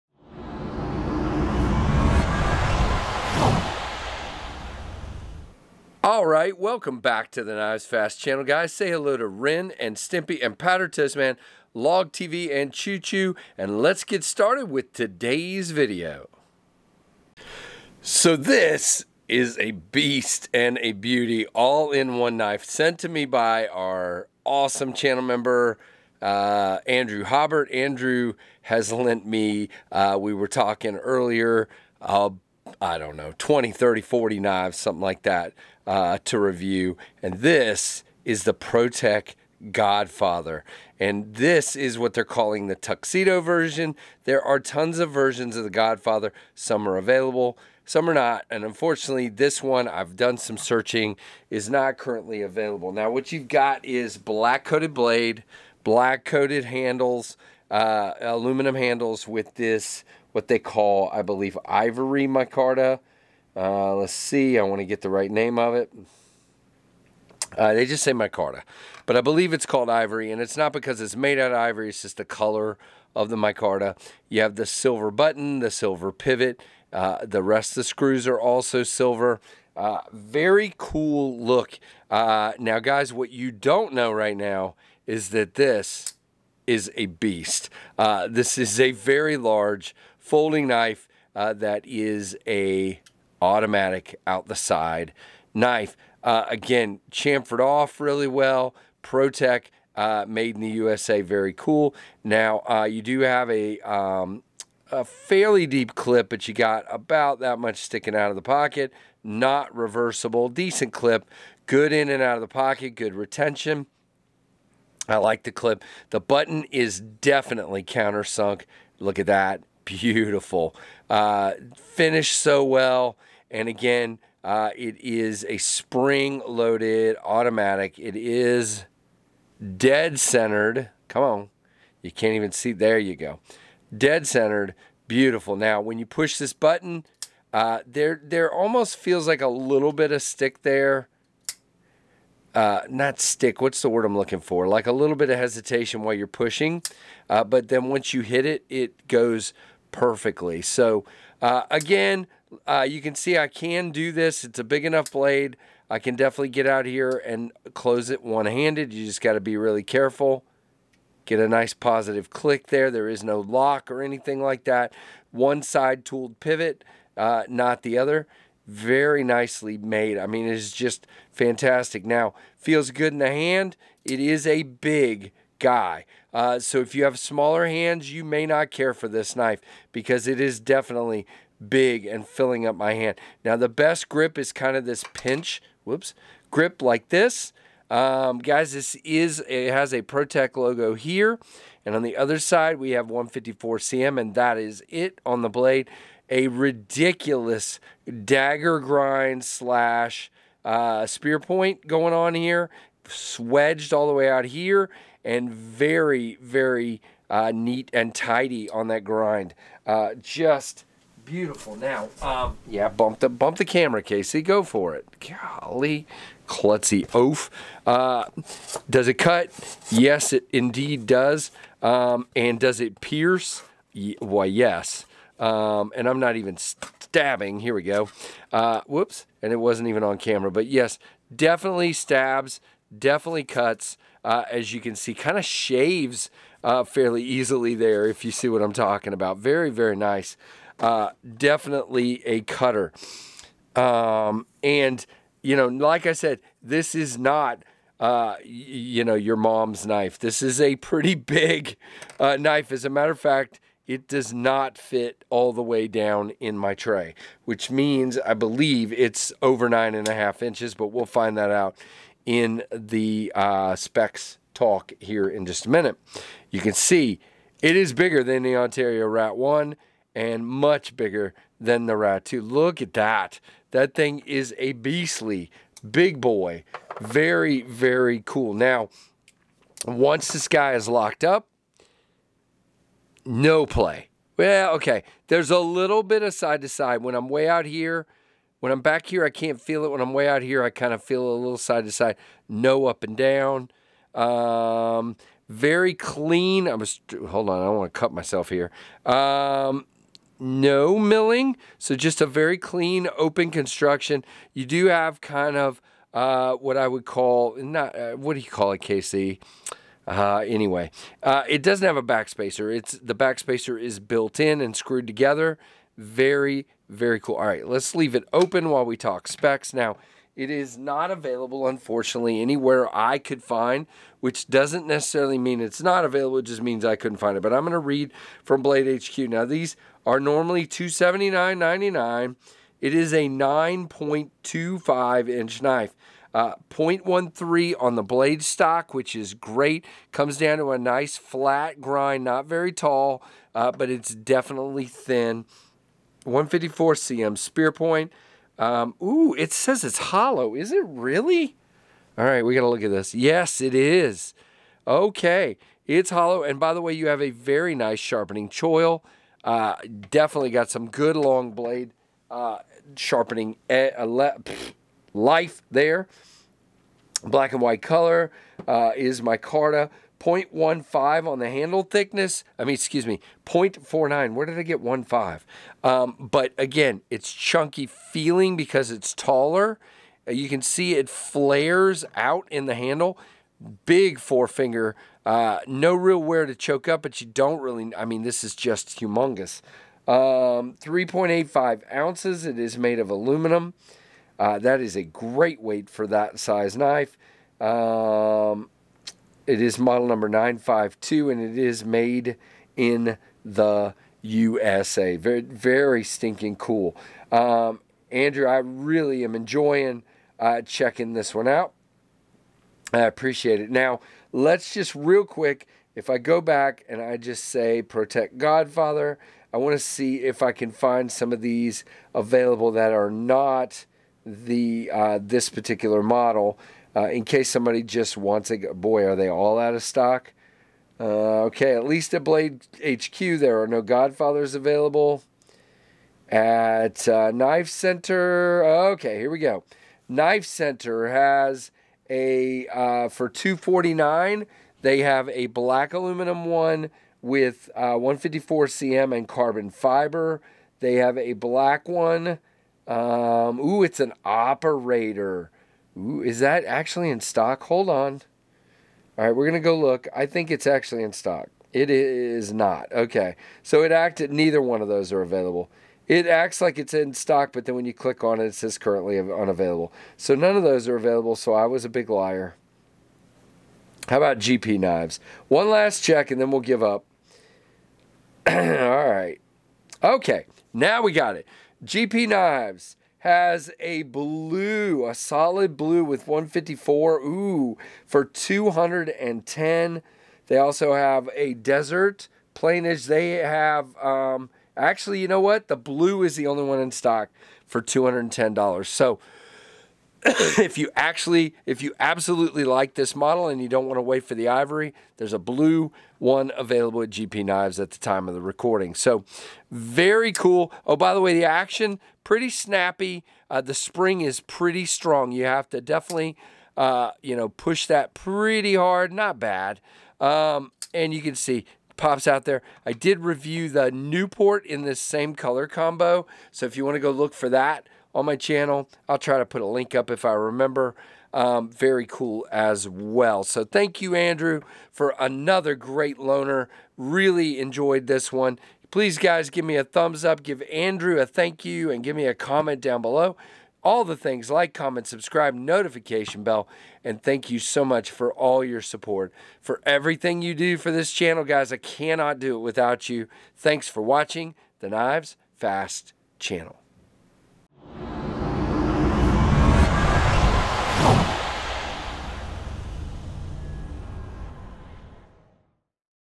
This one is a Stunning Beast!
This is my review of the Protech Godfather loaned to me by Andrew Haubert.